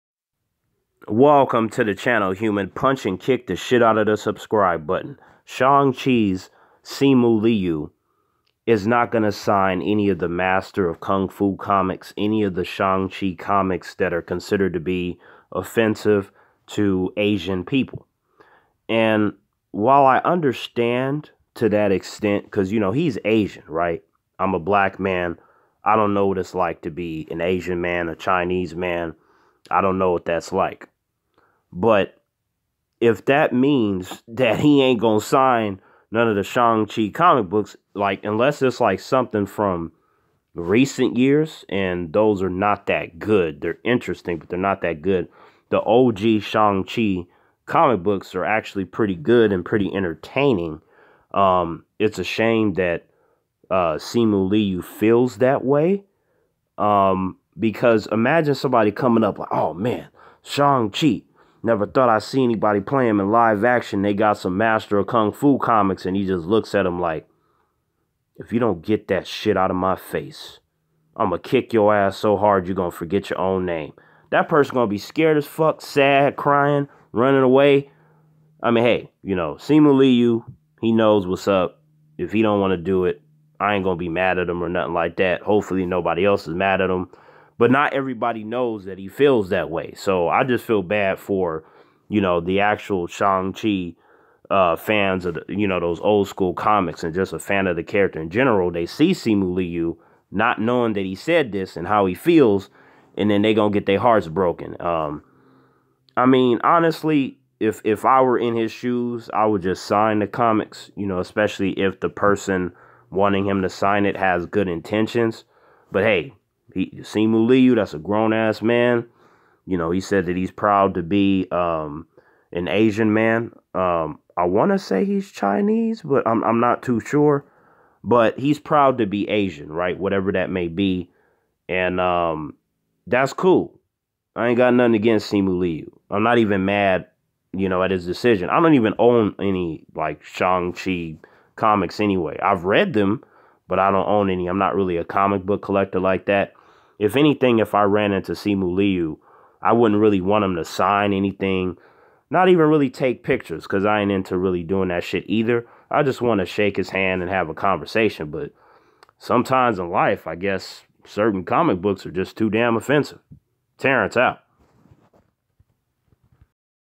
Shang-Chi's Simu Liu Refuses to Sign Master of Kung Fu Comics
While attending Awesome Con 2022, Shang-Chi actor Simu Liu will not be signing any Master of Kung Fu comics, the character's original comic title.